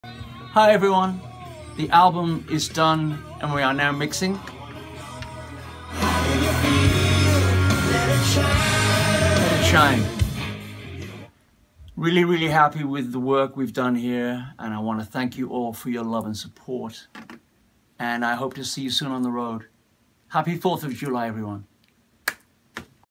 Message from Glenn Hughes - July 4th, 2016
Hi Everyone,

The album is done and we're now mixing… let it shine. Really, really happy with the work we've done here and I want to thank you all for your love and support and I hope to see you soon on the road.